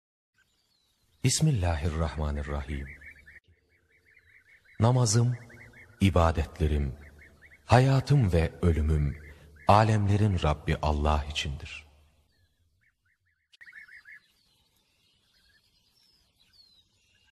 Kur'an-ı Kerim: En'âm: 162) Türkçe Meal..
'              Bismillâhirrahmânirrahîm. 💖

     De ki; Namazım ibadetlerim hayatım ve
     ölümüm Alemlerin Rabbi ALLAH içindir.

         (Kur'an-ı Kerim: En'âm, 162. Ayet) 💖